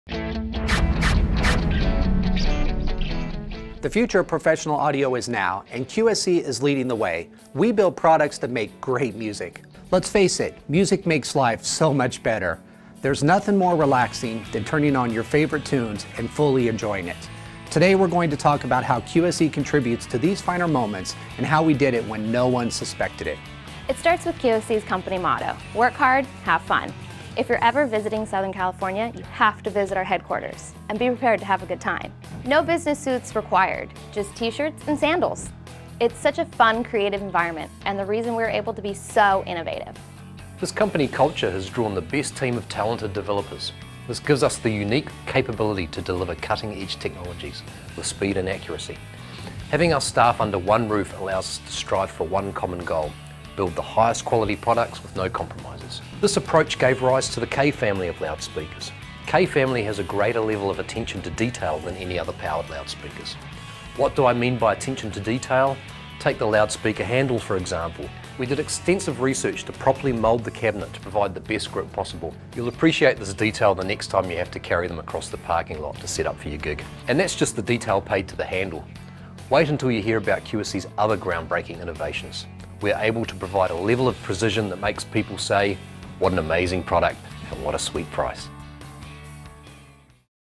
House of K for Rental Sound- Introduction
This videos describes QSC's culture of innovation and how it led the to the creation of the K Family of loudspeakers.

Providing quality sound reinforcement is your business. Whether your focus is large-scale productions or simple systems for the neighborhood block party, the fact remains that your business only makes money when its inventory is being rented. The QSC K Family of products (with 10 models to choose from) is designed to offer users unparalleled performance in flexible, easy-to-use configurations. Ideal for a broad range of applications, K Family loudspeakers also feature long-term durability and a clean, professional appearance that is at home in any situation. With K Family loudspeakers in your rental inventory, the next time a customer asks, "Have you got something that will...?" your answer will undoubtedly be YES!